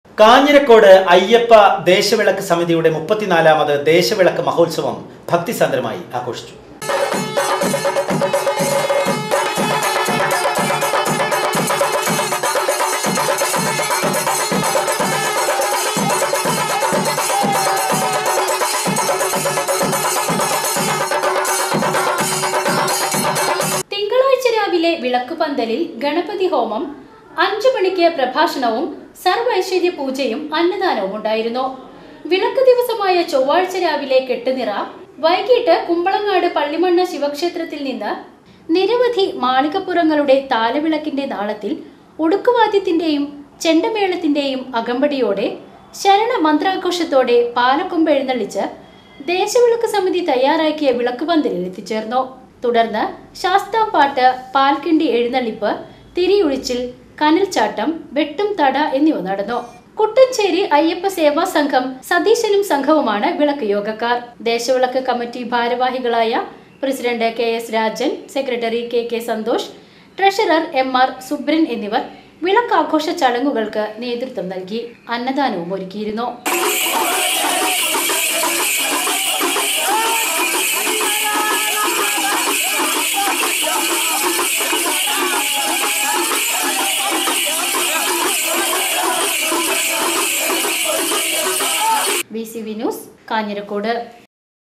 കാഞ്ഞിരക്കോട് അയ്യപ്പ ദേശ വിളക്ക് മഹോത്സവം ഭക്തി സാന്ദ്രമായി ആഘോഷിച്ചു
കാഞ്ഞിരക്കോട് അയ്യപ്പ ദേശവിളക്ക് സമിതിയുടെ 31 മത്  ദേശ വിളക്ക് മഹോത്സവം ഭക്തി സാന്ദ്രമായി   ആഘോഷിച്ചു

VCV NEW

Wadakkanchery, Thrissur